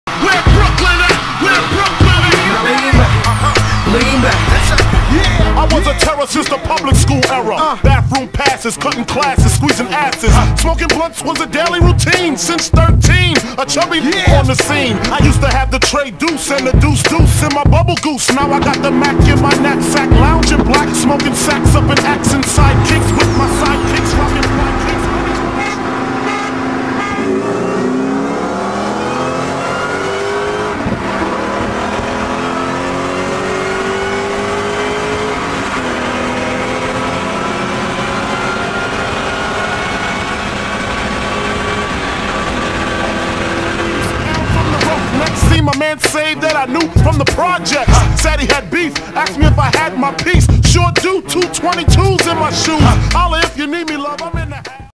N2S: BluMachine's Z06 vs. Mofobucs' Terminator Cobra!
Over 800rwhp doing battle in The Bay!  This is before the Z06 had spray, and before Mofobucs was even Mofobucs :-)

Could it be a Vargas Stage 2+ from Tune+?  Episode 2"